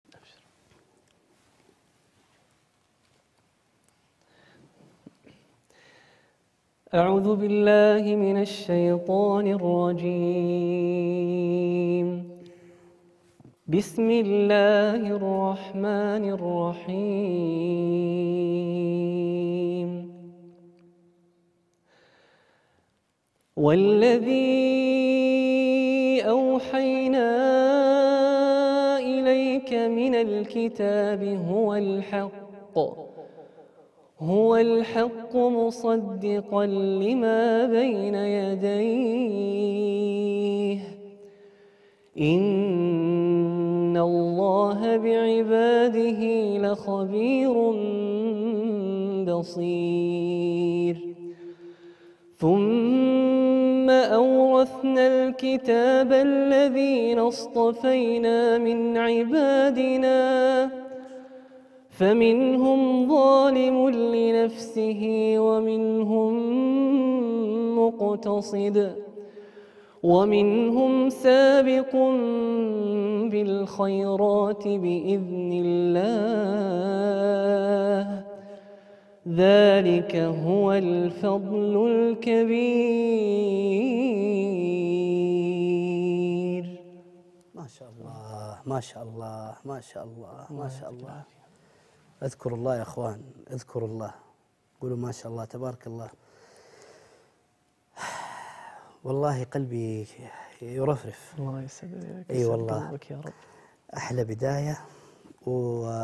تلاوة عطرة بصوت مؤثر لآيات من سورة فاطر | فاروق الجابري
كل سبت 9:30م
تقديم الحلقة:
جهاد اليافعي

ضيف الحلقة:
أ. أسامة الصافي

نسعد بخدمتكم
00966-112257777